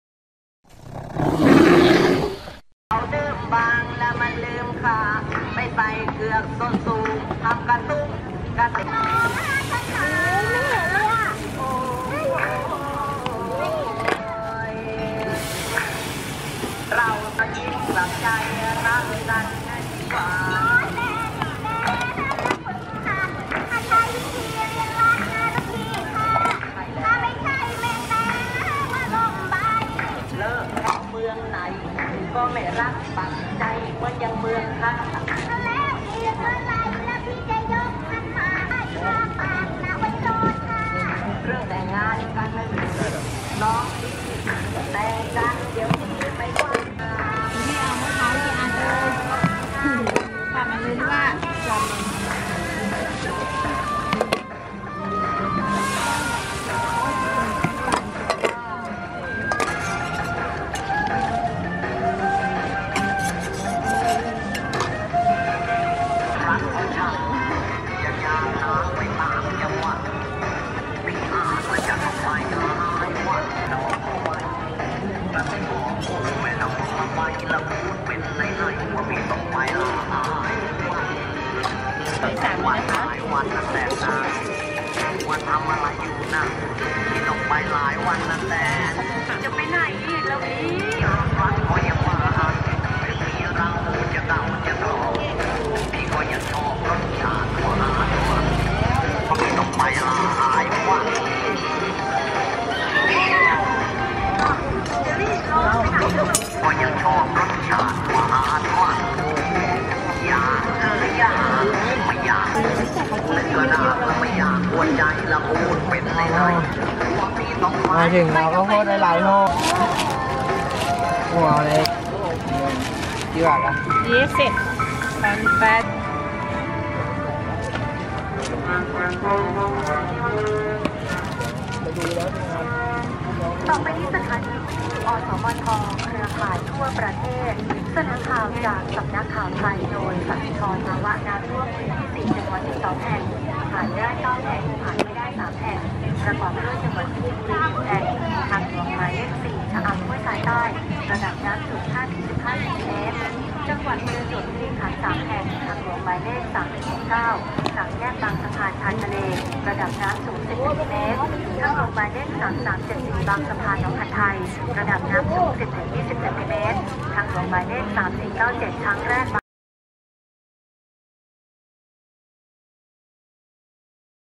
Thai Dessert (Kanom Tung Tak) | Thailand Street Food - Hungry Bear
Try Thai Dessert!!
Kanom tung tak means a dessert for poor people becaues it’s cheap as it’s mainly made of flour and sugar. Nowadays is a lot more than just flour and sugar. Sellers normally add Custard cream, coconut meat and sometimes sliced corn as well.